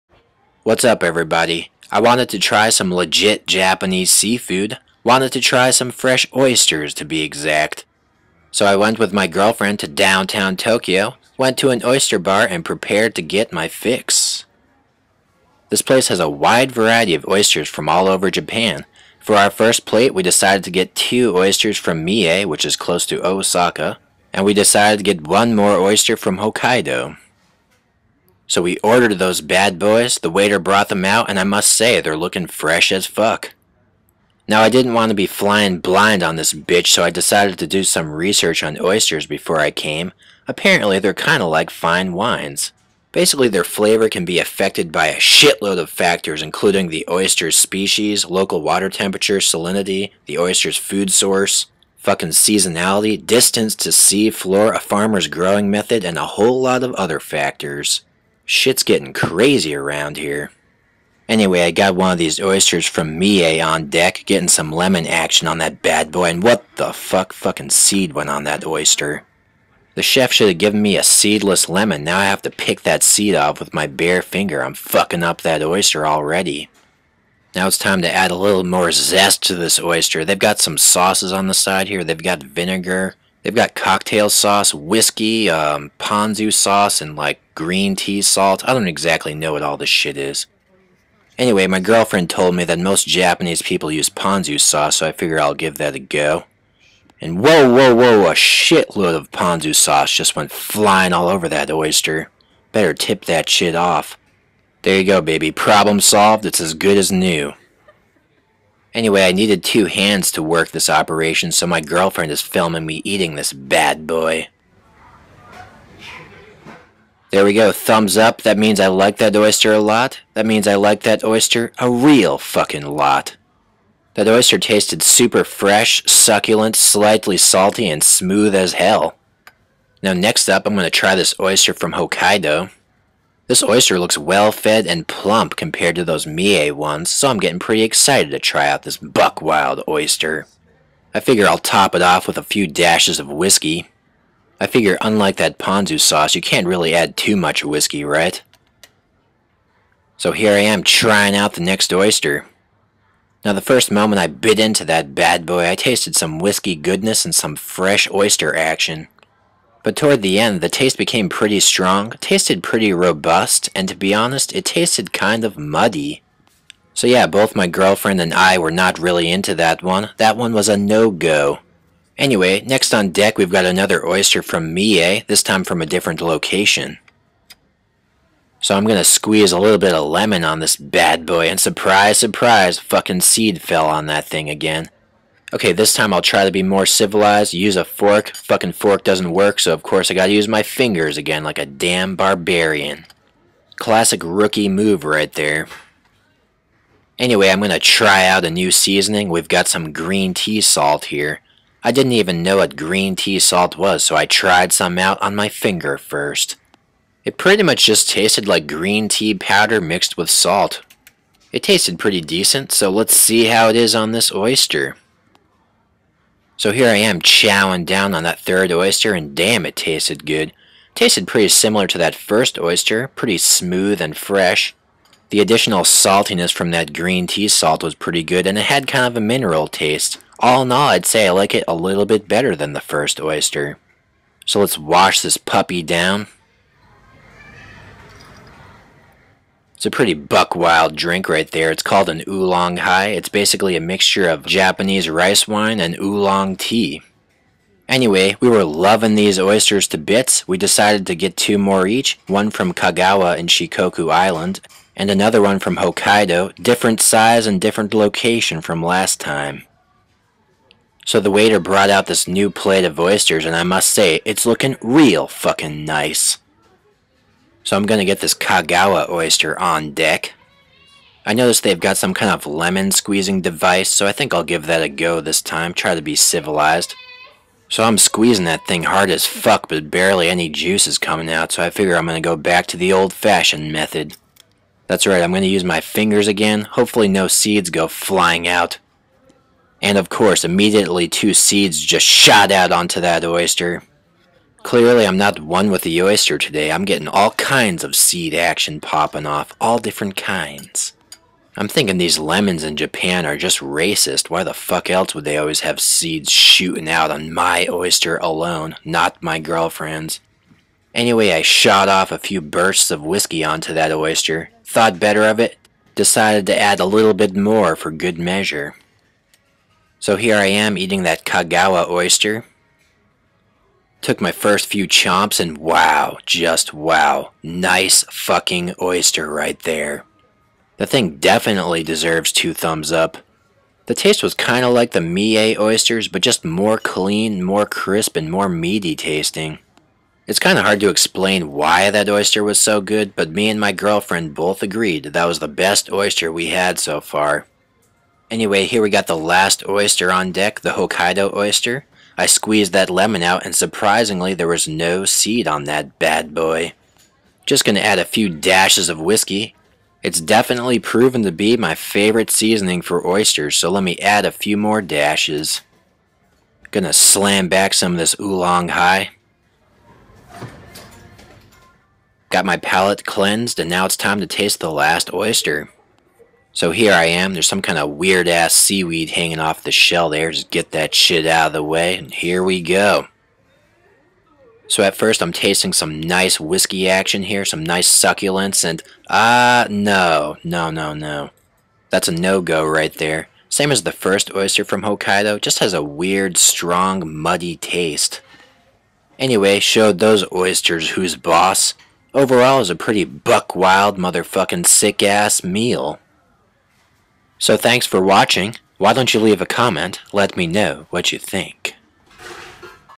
OYSTER BAR in Japan serves WHAT???
This oyster bar restaurant in Japan has FRESH giant oysters served raw. BUT you won't believe what they REALLY serve. Watch to the end to find out!

Name: Oyster Bar Jackpot, Shibuya
Address: 宇田川町15-1, パルコパート1 8F, Shibuya, 東京都 〒150-0042, Japan (Jinnan, Shibuya Station Hachiko-guchi, Shibuya Station)